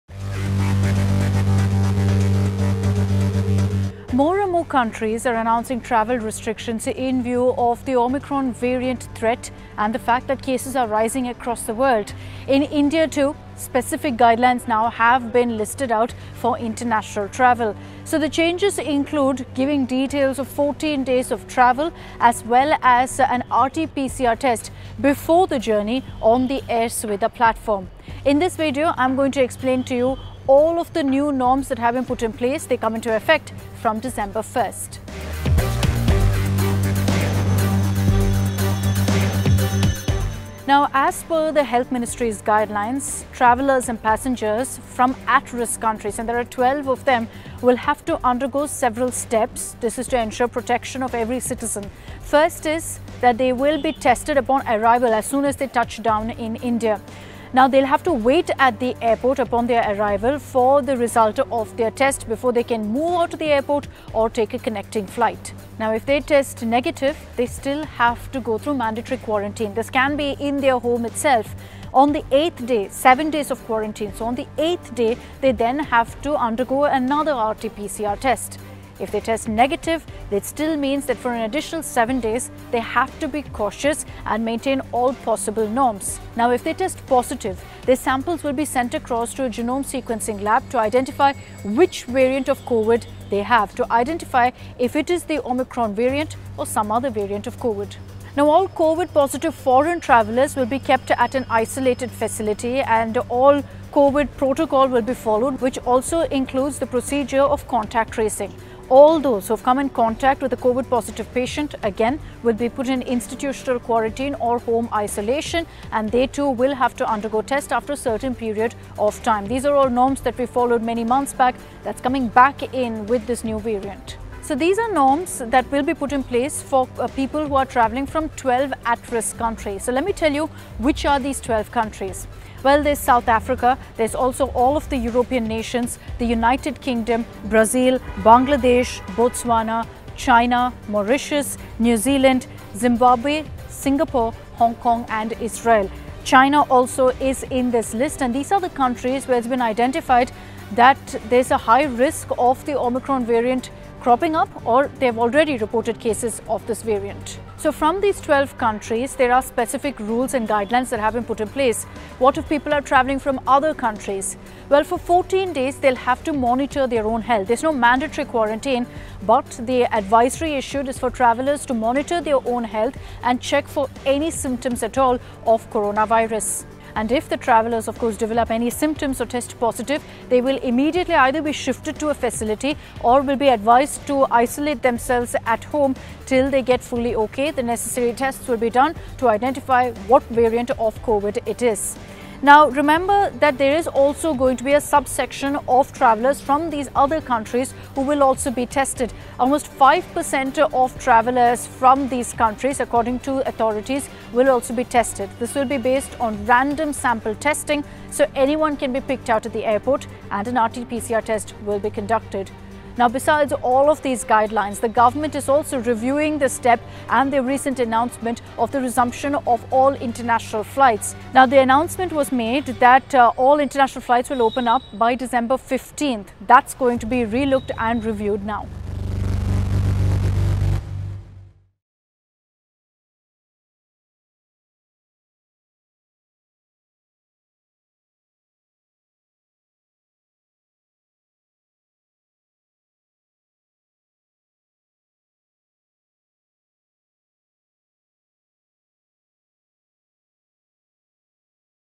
Omicron Scare: Fresh Rules For International Travellers To India | Decoded | India Today
Omicron Variant: The Union Health Ministry has issued new guidelines for international travelers to India from countries considered at-risk and even those that are seen as not at risk.